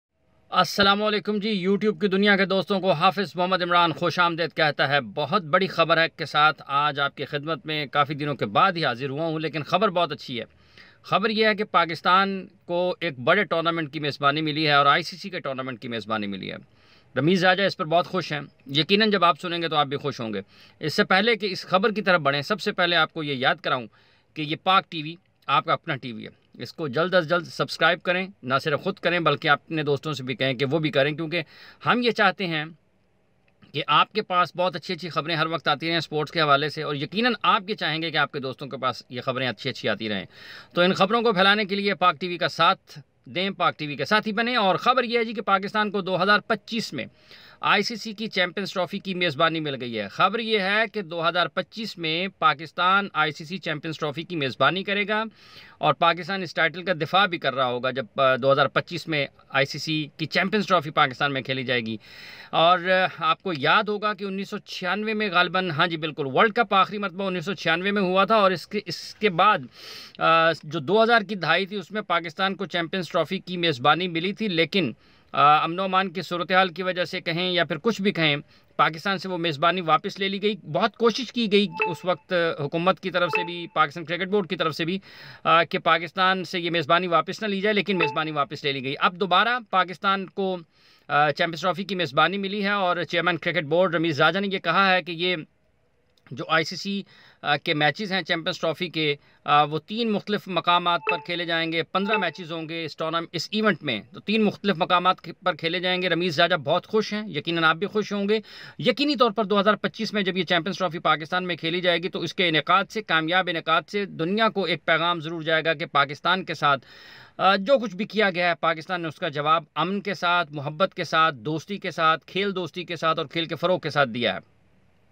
Pakistan will host Champions Trophy
International Cricket council has announced future plan for Champions Trophy and Pakistan will host 2025 champions trophy event.